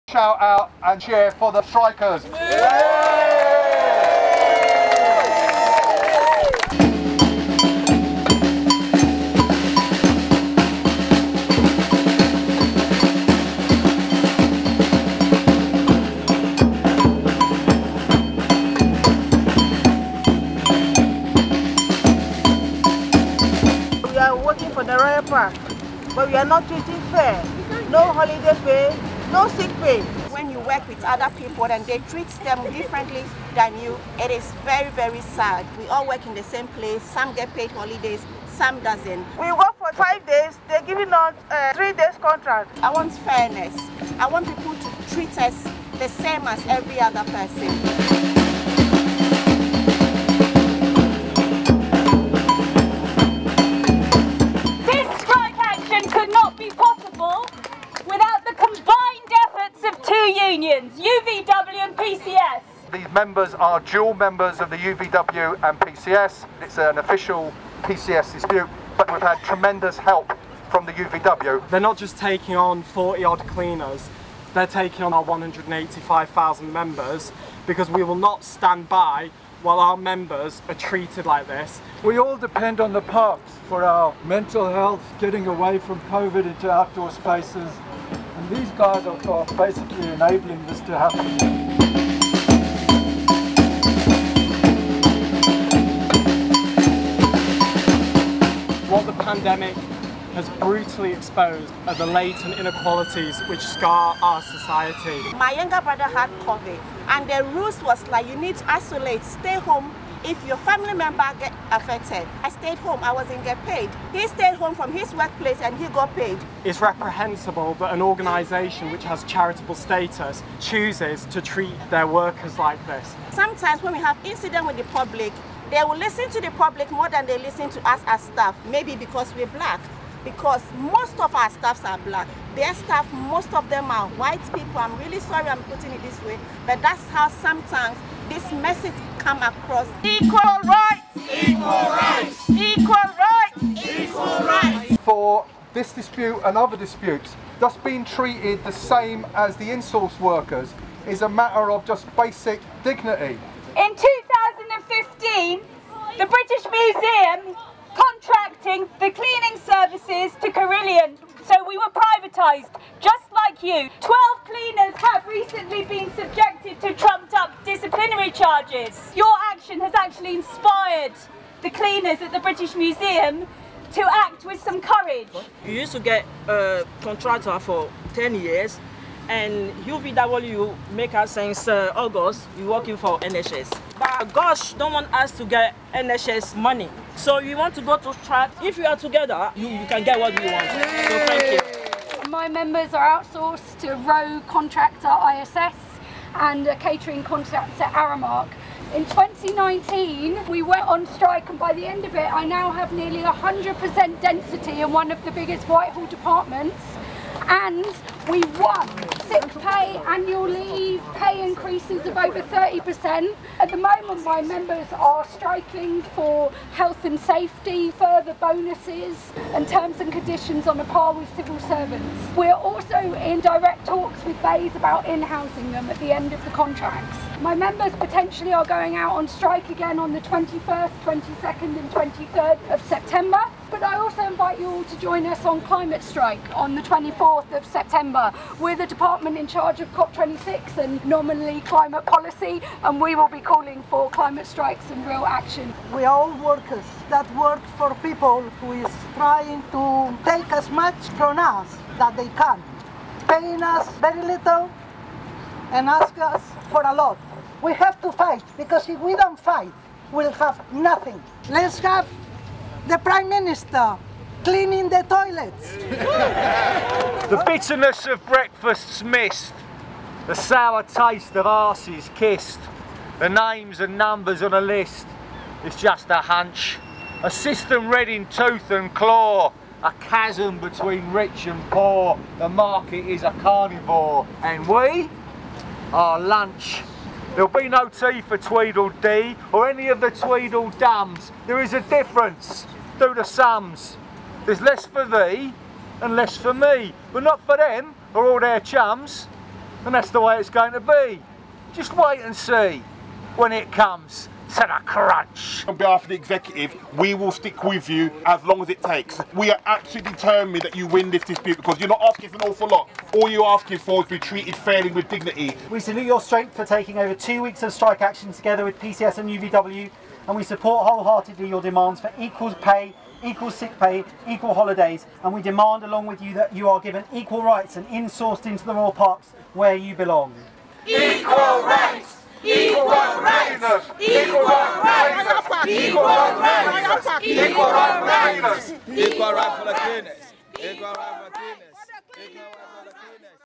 Royal Parks key workers demand equality and respect
On bank holiday Monday, cleaners and attendants working in the Royal Parks of London completed a two week strike with a rally and march through Hyde Park and Kensington Park.

The workers - outsourced to Just Ask Estate services - have far worse conditions that workers directly employed by Royal Parks. They have less holiday, are only on three day week contracts despite working five days a week - and, disgracefully for workers who are playing such a key role during the pandemic, they don't even get any sick pay.

If Royal Parks don't rectify this appalling situation more strikes will follow. Next strike: 1-31 October; donate to the strike fund by scanning the code at the end of the video. And for updates on next steps go to pcs.org.uk.